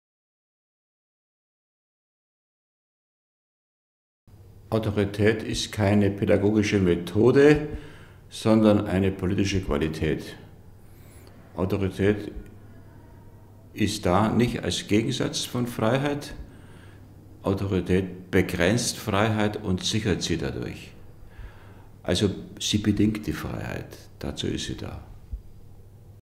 Peter Gauweiler
Video-Statement von Peter Gauweiler zum Thema Autorität beim NZZ Podium Berlin am 20. März 2019.